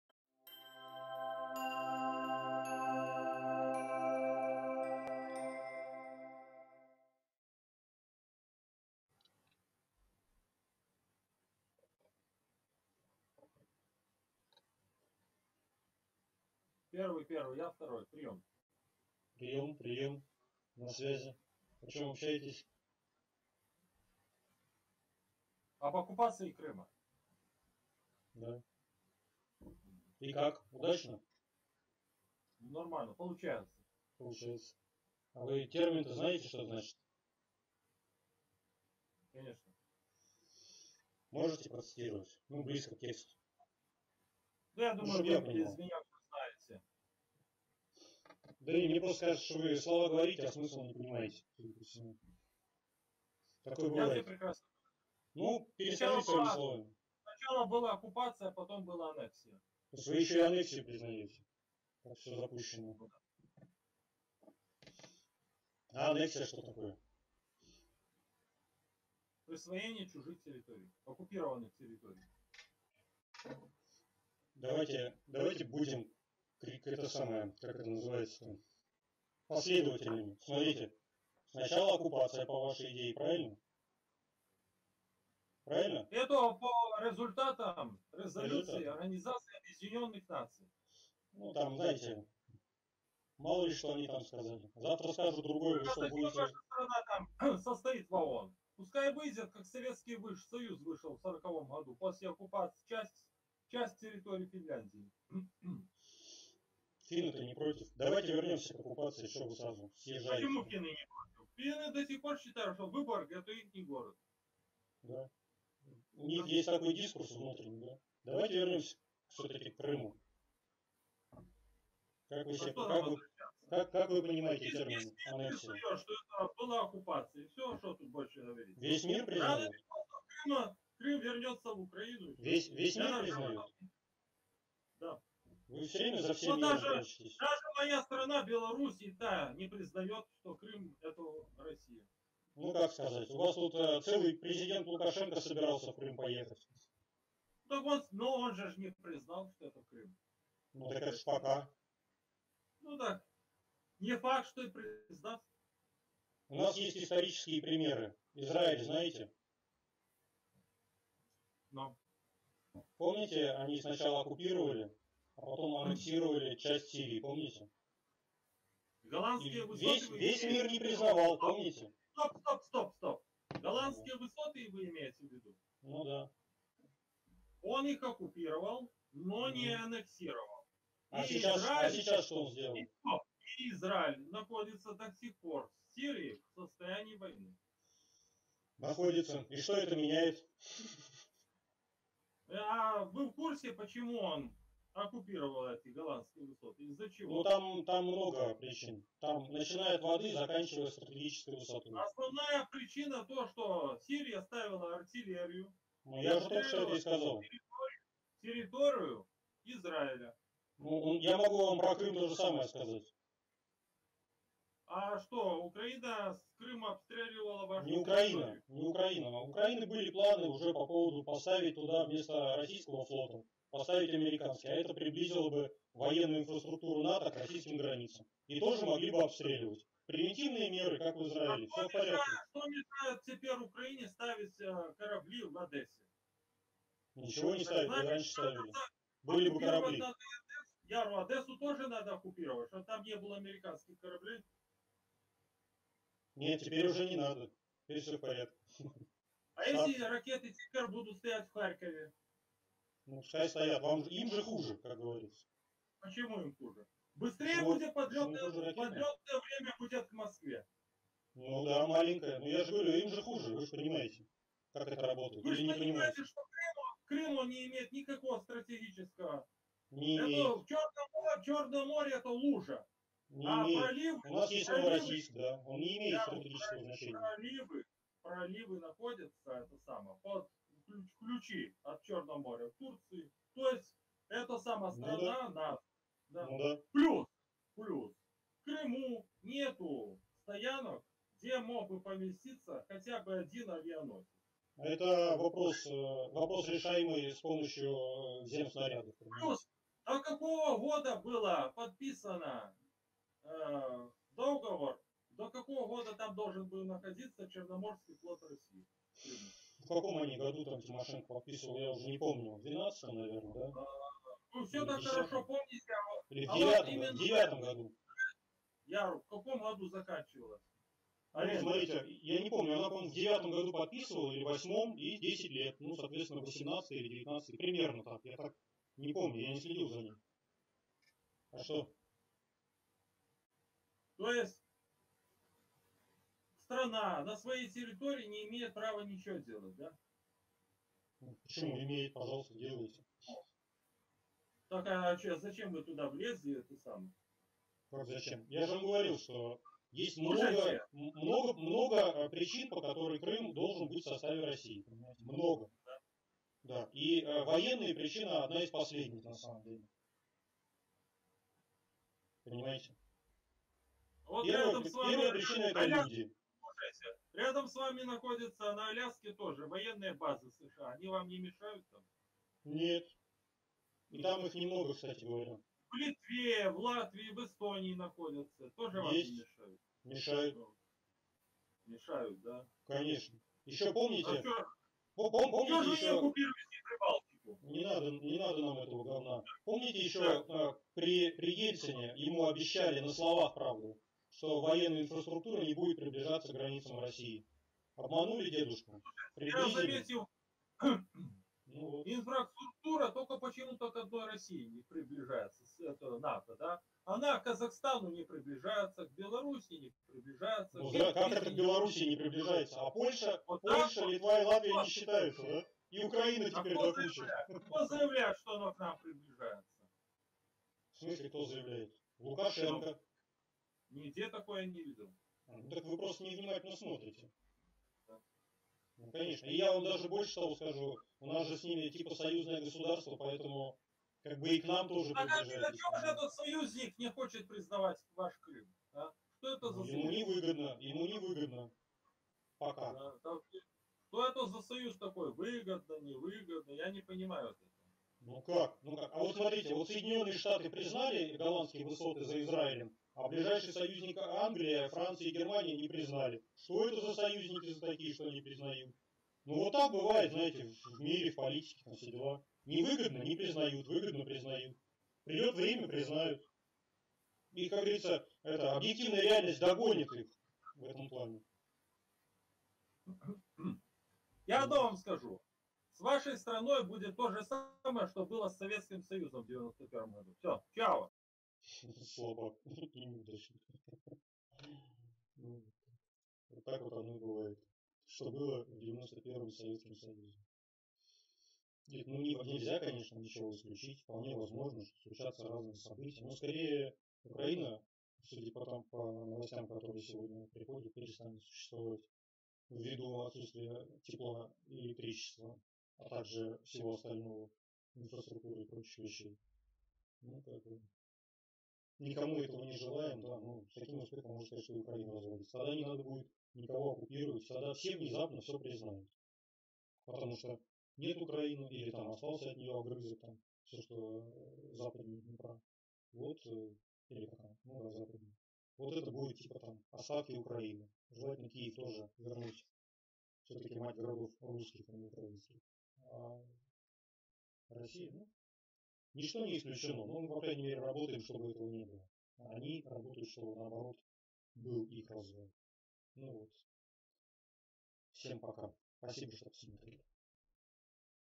#305 Я хоть и из Белоруссии, но Россия оккупировала крым (звук не очень)
накидали много тем, и в каждой для собеседника нашелся неудобный факт...